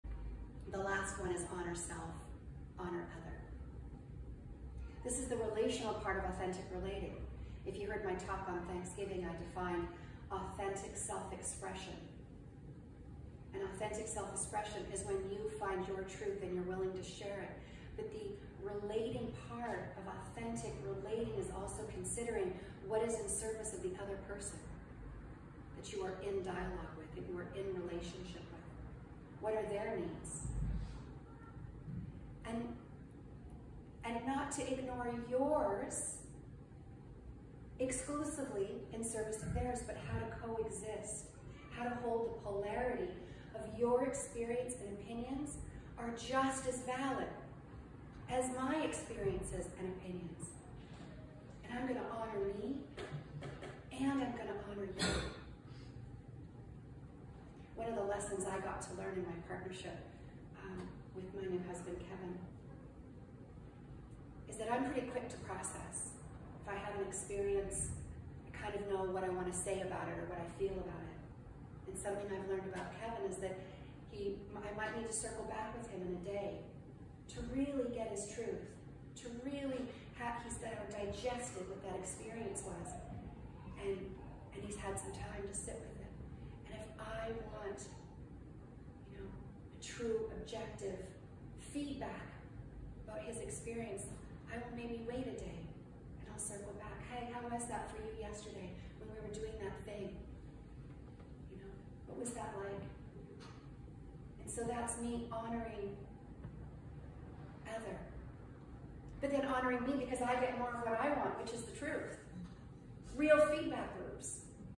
ART Practices: Honor Self / Honor Other
ART = Authentic Relating Training. Course Leader, Sheri Hauser was giving a talk at Victoria Truth Centre, sharing her thoughts and feelings about the power of these practices. In this clip, she's describing Practice #5, Honor Self / Honor Other